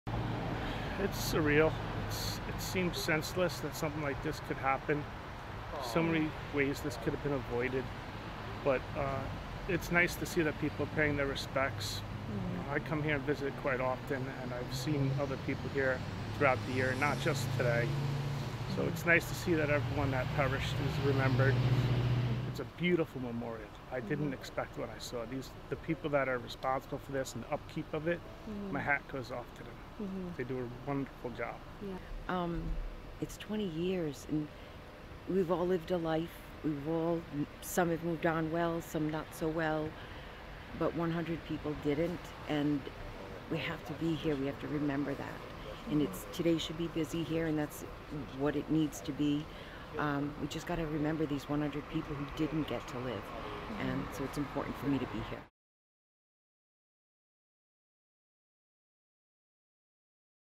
VIDEO NOW: Rhode Islanders remember those lost in the Station Nightclub Fire
On the 20th anniversary of the tragic fire, many gather at the local memorial park to pay respects to those who lost their lives.